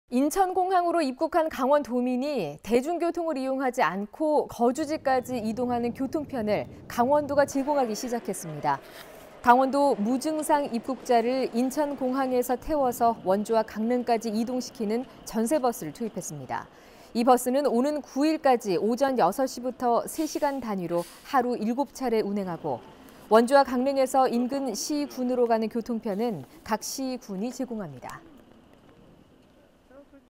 2020. 4. 3 [원주MBC] 도, 인천공항 귀국 도민 수송버스 투입
인천공항으로 입국한 강원도민이
대중교통을 이용하지 않고
거주지까지 이동하는 교통편을
강원도가 제공하기 시작했습니다.

강원도 무증상 입국자를 인천공항에서 태워
원주와 강릉까지 태워 나르는 전세버스를
투입했습니다.

이 버스는 오는 9일까지 오전 6시부터
3시간단위로 하루7차례 운행하고,
원주와 강릉에서 인근 시군으로 가는
교통편은 각 시군이 제공합니다.